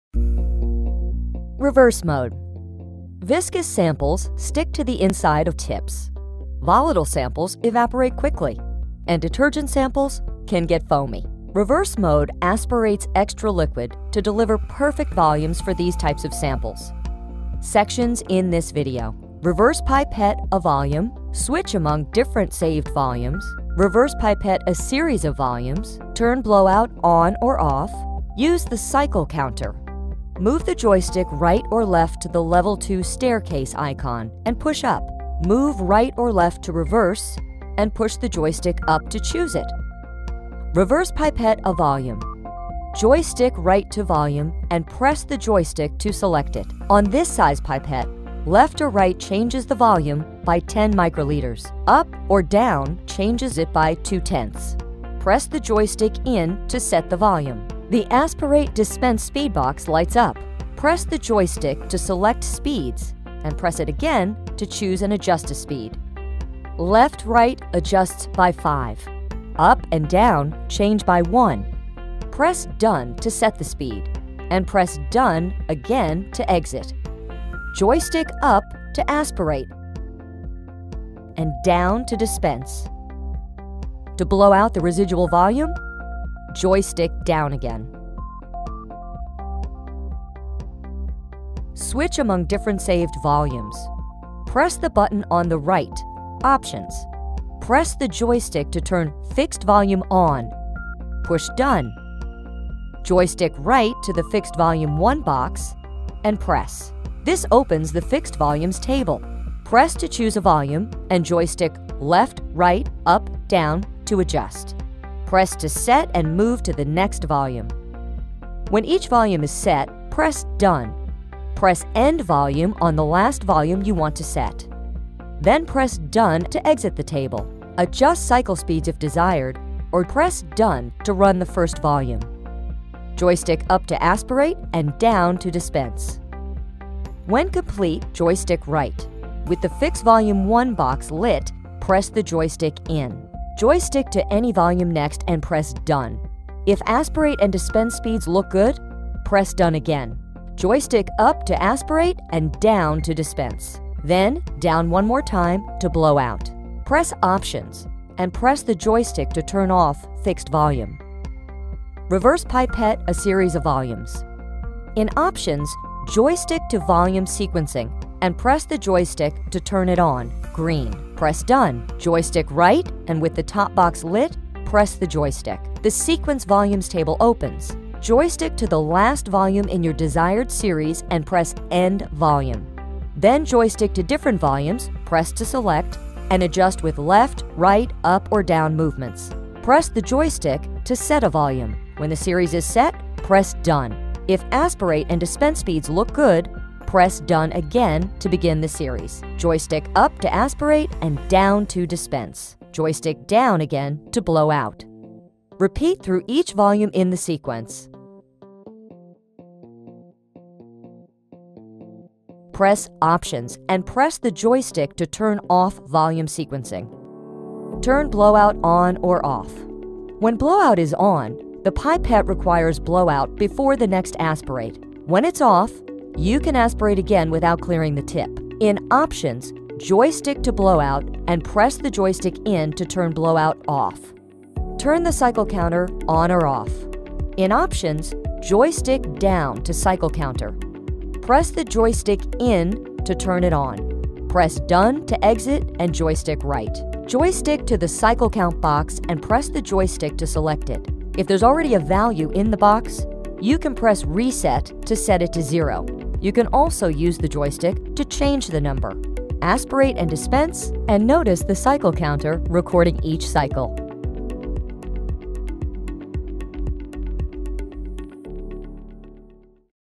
Electronic Pipette | Rainin E4 | Reverse Mode
Rainin E4 XLS and XLS+ Electronic pipettes
Joystick-controlled, with different liquid-end versions: Single channel, Multichannel and Adjustable Spacer.  Volume ranges from 0.5 µL - 20 mL.

Reverse mode

Viscous samples stick to the inside of pipette tips. Volatile samples evaporate quickly, and detergent samples can get foamy.

Reverse mode aspirates extra liquid to deliver perfect volumes for these types of samples.

Sections in this video:
- Reverse-pipette a volume
- Switch among different saved volumes
- Reverse-pipette a series of volumes
- Turn Blowout On or Off
- Use the cycle counter

Move the joystick right or left to the Level Two "staircase" icon and push up. Move right or left to Reverse, and push the joystick up to choose it.

REVERSE PIPETTE A VOLUME

Joystick right to volume, and press the joystick to select it.

On this-size pipette, left or right changes the volume by 10 microliters.
Up or down changes it by two-tenths.

Press the joystick in to set the volume.

The aspirate / dispense speed box lights up.
Press the joystick to select speeds, and press it again to choose and adjust a speed.
Left-right adjusts by 5. Up and down change by one.
Press DONE to set the speed, and  press DONE again to exit.

Joystick up to aspirate, and down to dispense. To blow out the residual volume, joystick down again.

Press the button on the right: OPTIONS.

Press the joystick to turn Fixed Volume ON – green. Push DONE.

Joystick right to the "Fixed Volume 1" box, and press. This opens the Fixed Volumes table.

Press to choose a volume, and joystick left-right-up-down to adjust. Press to set and move to the next volume.

When each volume is set, press DONE. Press END VOLUME on the last volume you want to set. Then press DONE again to exit the table.

Adjust cycle speeds if desired, or joystick Left to run the first volume.

Joystick up to aspirate, and down to dispense.

When complete, joystick right. With the "Fixed Volume 1" box lit, press the joystick in.

Joystick to your next volume and press DONE to exit. Press DONE again if aspirate and dispense speeds are already set.

Joystick up to aspirate, and down to dispense. Joystick down again to blow out.

Joystick right. On the "Fixed Vol 1" box, press the joystick.

Joystick to any volume next, and press DONE. If aspirate and dispense speeds look good, press DONE again.

Joystick up to aspirate, and down to dispense. Then down one more time to blow out.

Press OPTIONS, and press the joystick to turn off Fixed Volume.

In OPTIONS, joystick to Volume Sequencing and press the joystick to turn it on – green.

Press DONE. Joystick right and with the top box lit, press the joystick. The sequence volumes table opens.

Joystick to the last volume in your desired series and press END VOLUME.

Then joystick to different volumes, press to select, and adjust with left, right, up or down movements. Press the joystick to set a volume.

When the series is set, press DONE. If aspirate and dispense speeds look good, press DONE again to begin the series.

Joystick up to aspirate, and down to dispense. Joystick down again to blow out. Repeat through each volume in the sequence.

Press OPTIONS, and press the joystick to turn off Volume Sequencing.

When blowout is ON, the pipette requires blowout before the next aspirate. When it's off, you can aspirate again without clearing the tip.

In OPTIONS, joystick to Blowout and press the joystick in to turn blowout ON or OFF.

To clear the tip, press RESET.

TURN THE CYCLE COUNTER ON OR OFF

In OPTIONS, joystick down to Cycle Counter. Press the joystick in to turn it on.

Press DONE to exit, and joystick right.

Joystick to the Cycle Count box and press the joystick to select it. If there's already a value in the box, you can press RESET to set it to zero. You can also use the joystick to change the number.

Aspirate and dispense, and notice the cycle counter recording each cycle.

0:02 Why use Reverse pipetting
0:30 Reverse pipette a volume
1:32 Switch among saved volumes
2:41 Reverse pipette a series of volumes
3:40 Turn blowout on/off
3:57 Use the cycle counter